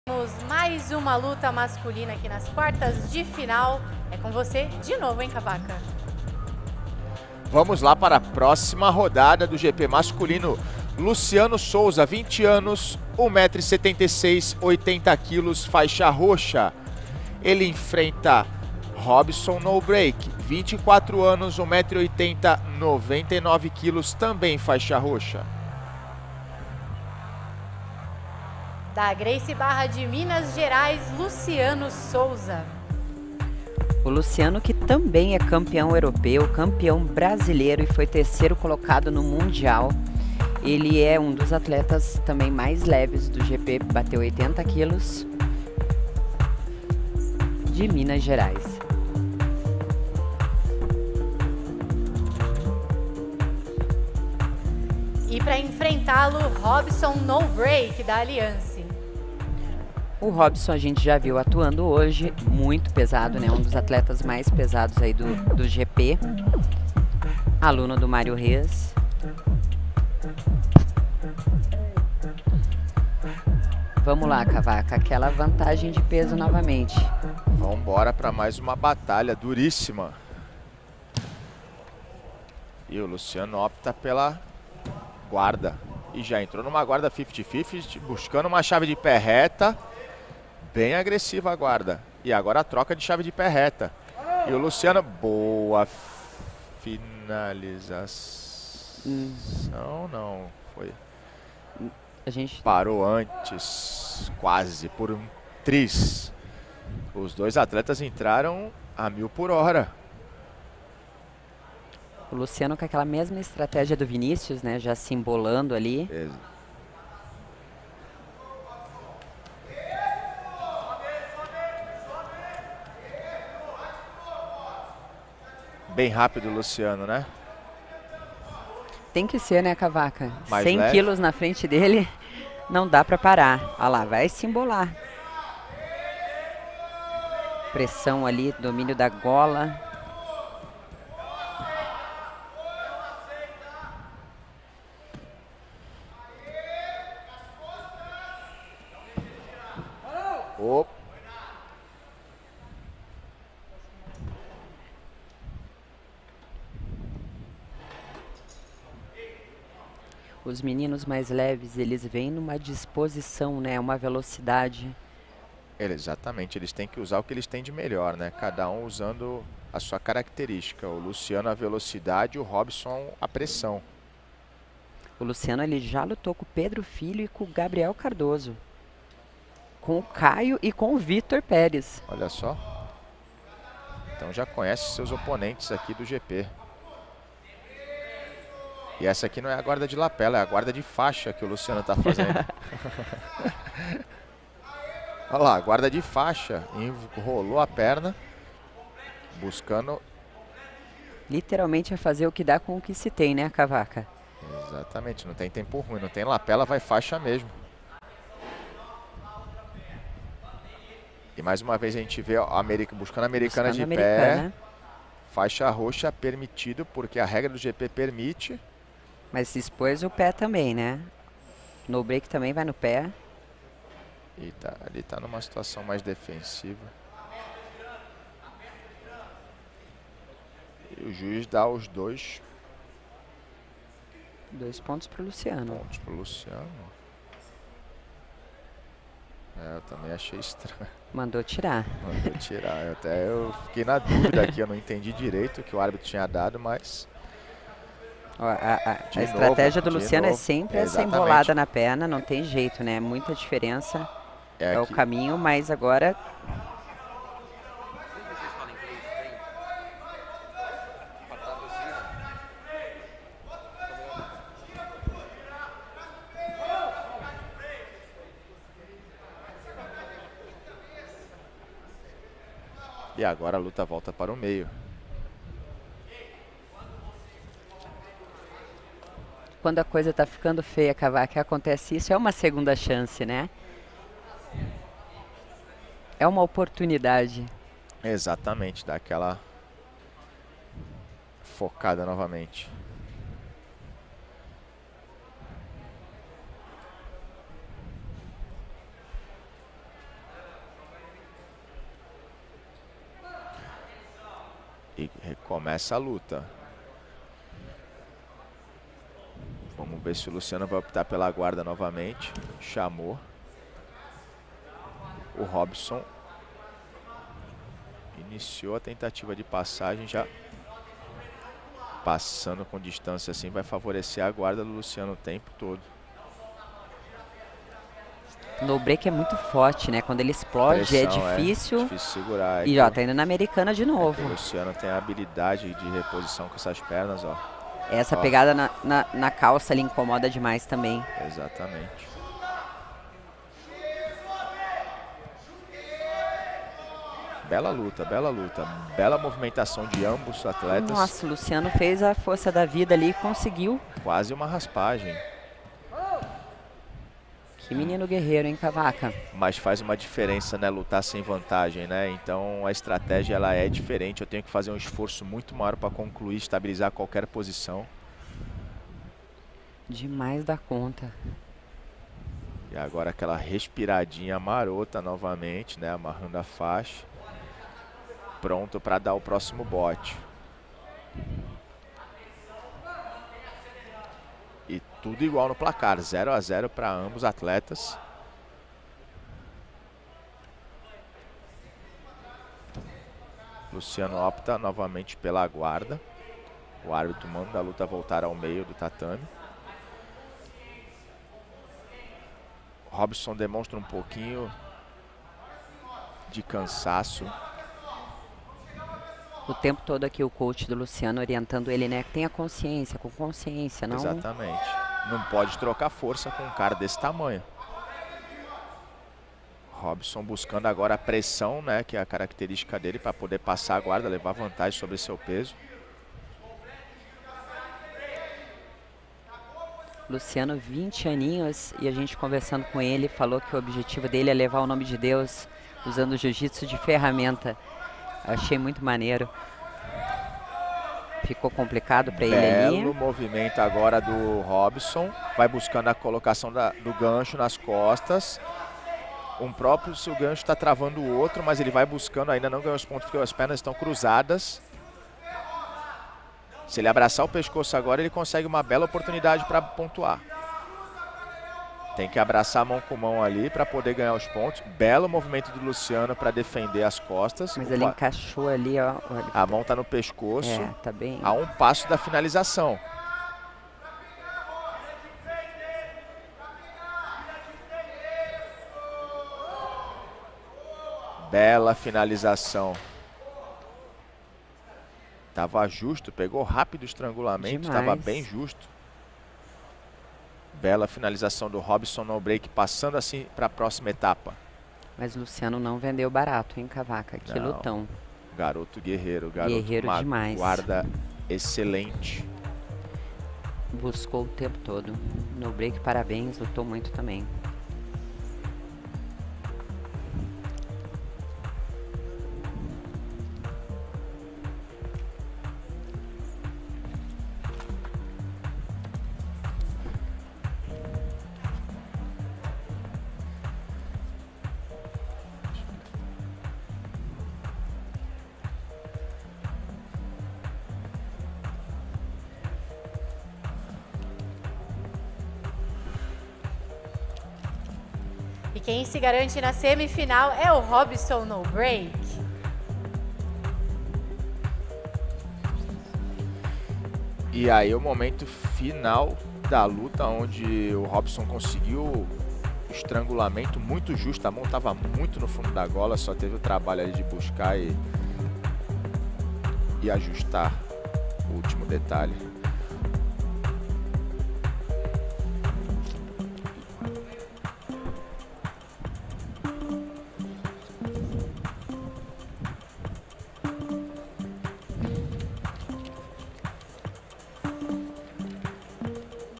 LUCIANO SOUZA X ROBSON NOBRAKE
Quartas de final - Luciano Souza x Robson Nobrake

Gp Masculino BJJBET SELECTION